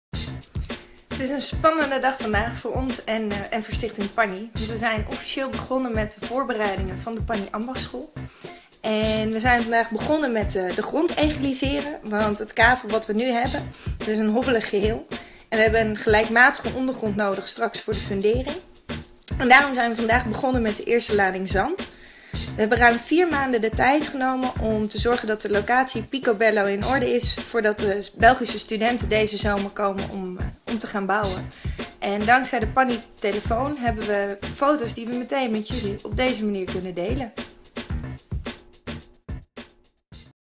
Pani bouw van start!
Hilde van Dijk - oprichter van Stichting Pani - vertelt over de start van de Pani Ambachtsschool!